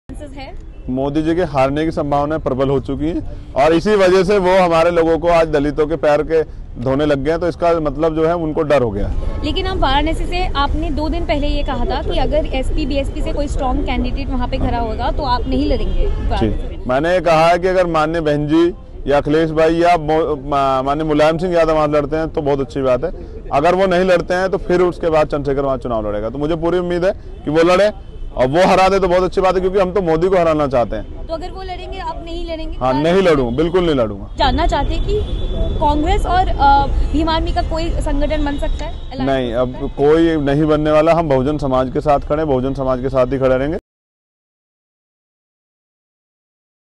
Mayawati-Akhilesh अगर PM Modi के खिलाफ नहीं लड़ें, तो मैं लडूंगा। Quint Hindi
केंद्र सरकार को घेरने के लिए दलित संगठन BhimArmy ने दिल्ली के जंतर-मंतर पर आज बहुजन हुंकार रैली बुलाई है. इस रैली में भीम आर्मी के चीफ चंद्रशेखर आजाद भी शामिल हुए. भीम आर्मी ने दलित नेता कांशीराम की जयंती के मौके पर इस बहुजन हुंकार रैली का आयोजन किया है.